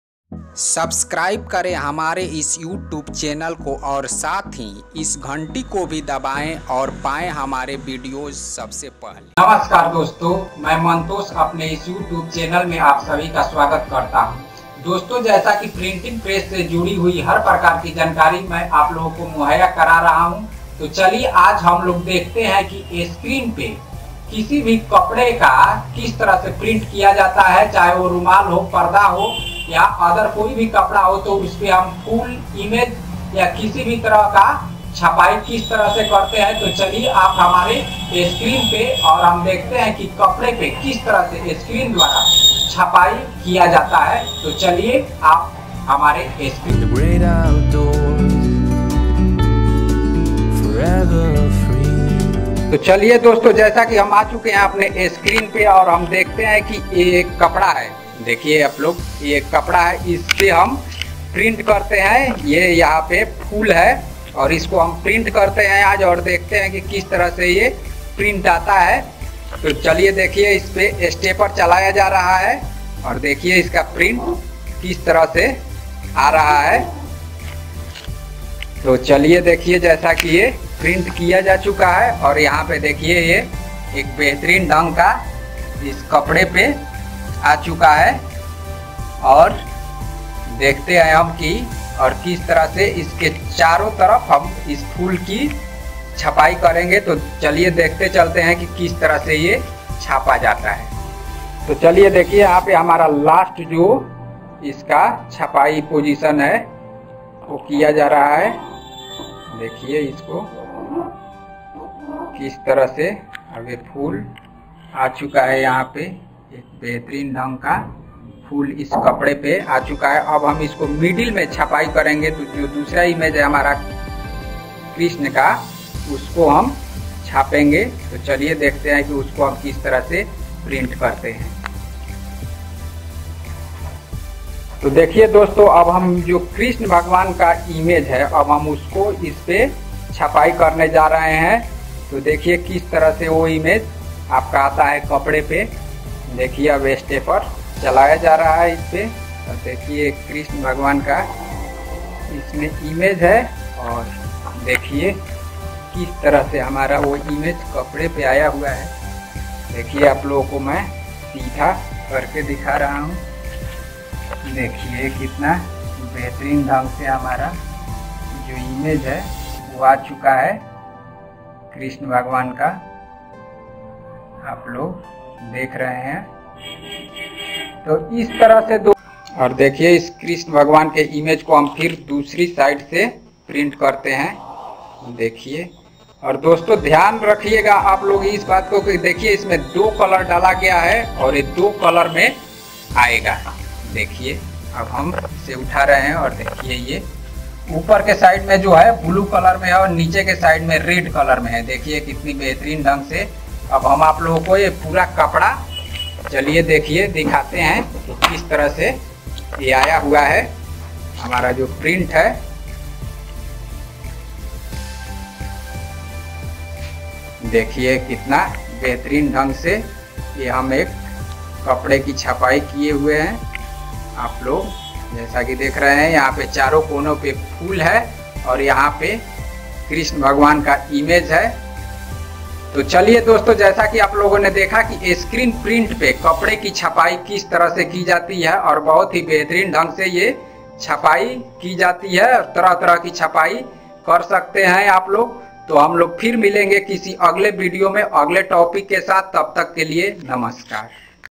clouth print in screen स्क्रीन प्रिंट पे कपडे की छपाई
नमस्कार दोस्तों इस वीडियो में आप लोग देखे की स्क्रीन पे किस तरह से कपडे की छपाई किया जाता है।